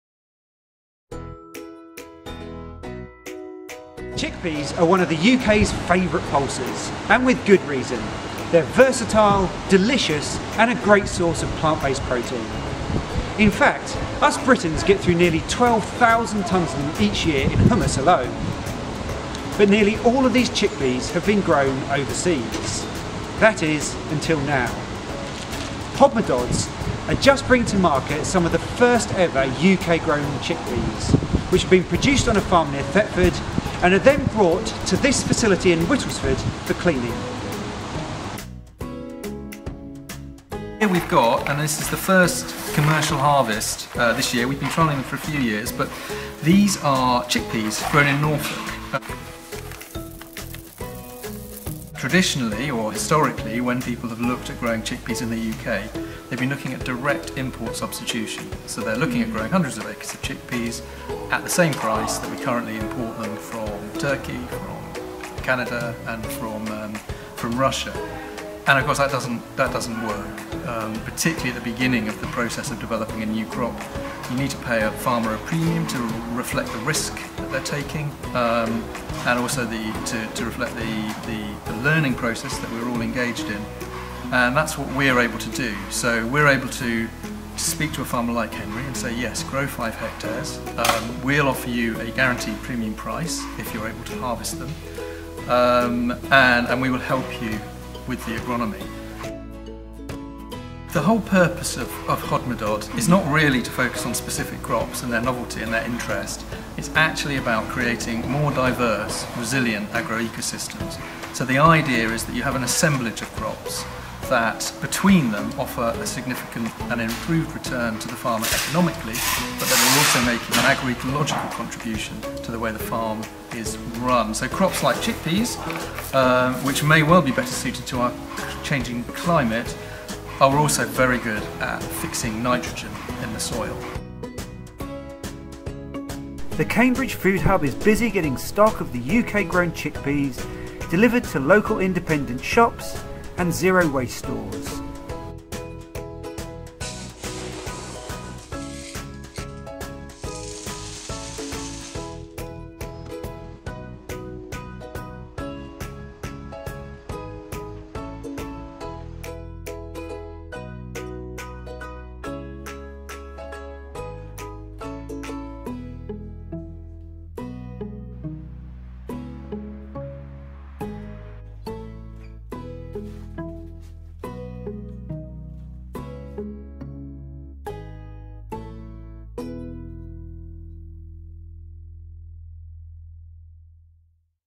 Chickpeas for a Changing Climate
Chickpeas are suited for a changing climate, provide a valuable source of plant-based protein and bring agroecological and economical benefits to farmers - is it any mystery why we're so excited about them?

Featuring Hodmedod, Organic Health Cambridge, Arjuna Wholefoods, The Larder at Burwash Food Hall, Radmore Farm Shop, Full Circle Shop UK.